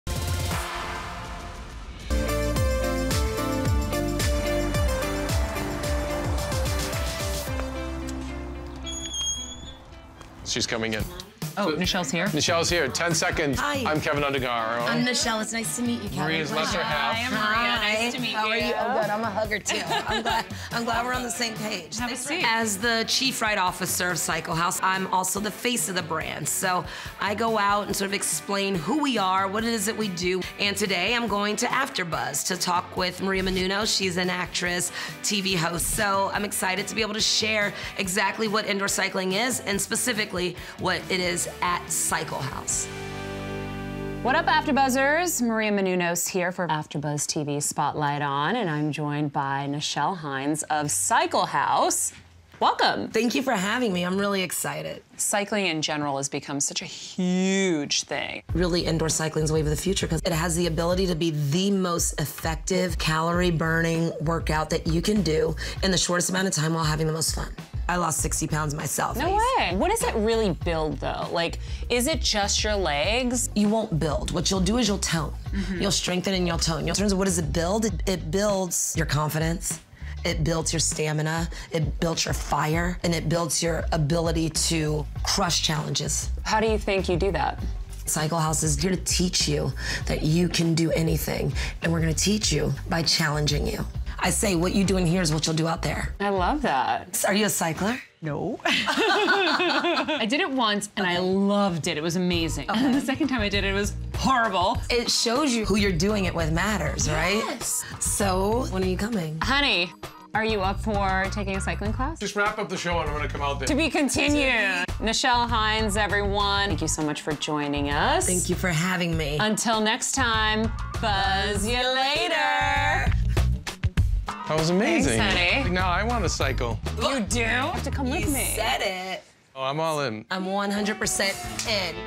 Nichelle Convinces Maria Menounos to Start Spinning | Hollywood Cycle | E!
The "Hollywood Cycle" star visits with E!'s Maria Menounos to promote Cycle House! Plus, find out how much weight Nichelle lost from spinning!